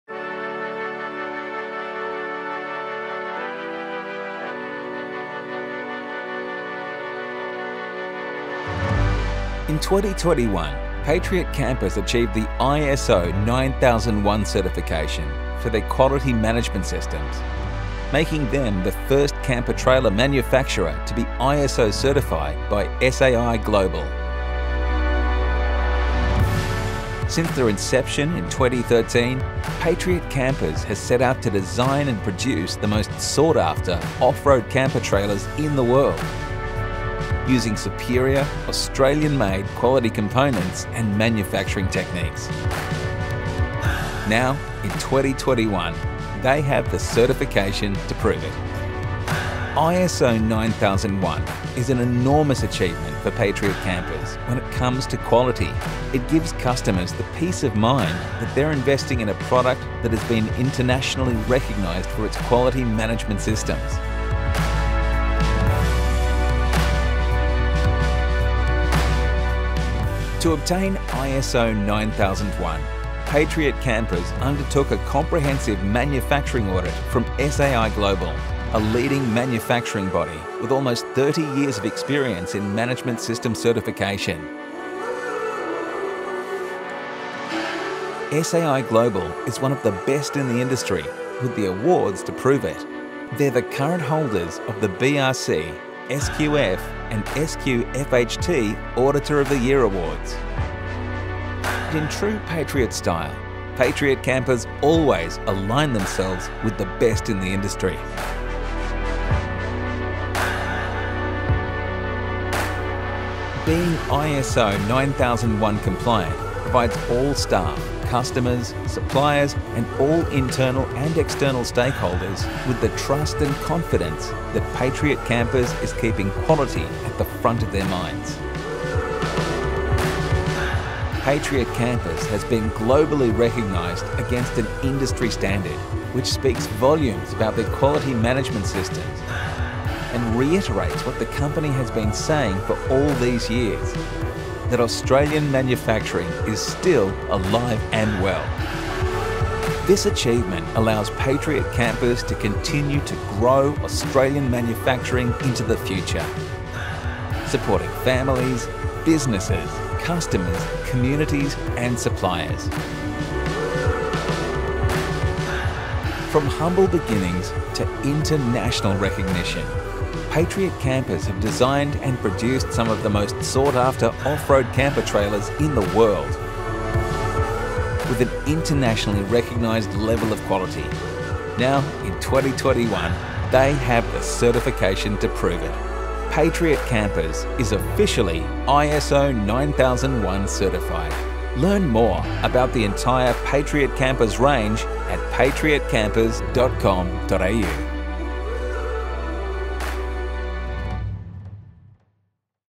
Our Quality Is Proven - Patriot Campers Are Now ISO 9001 Certified
Since our inception in 2013 we have set out to design and produce the most sought-after, off road camper trailers in the world, using superior, Australian made, quality components and manufacturing techniques.

And now we have the certification to prove it! This October Patriot Campers achieved the ISO 9001 certification, making us the first Australian Made camper trailer manufacturer to be ISO certified by SAI global.

The 5 Ticks StandardsMark™ certifies that Patriot Campers has undergone a rigorous audit and testing program.